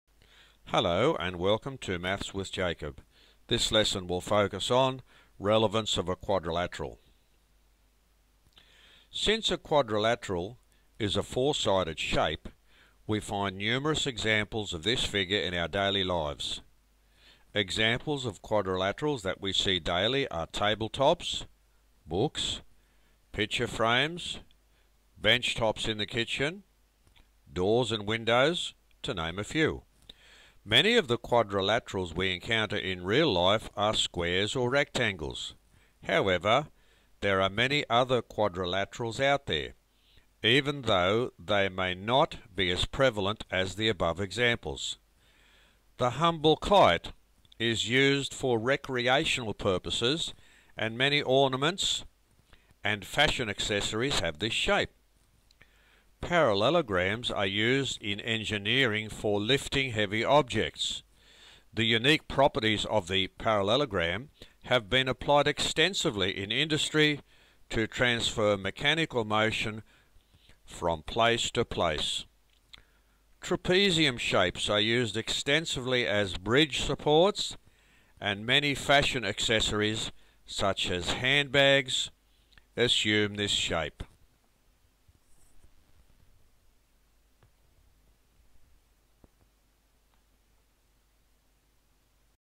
Relevance of the Quadrilateral
This video highlights some areas where the quadrilateral is used in real life.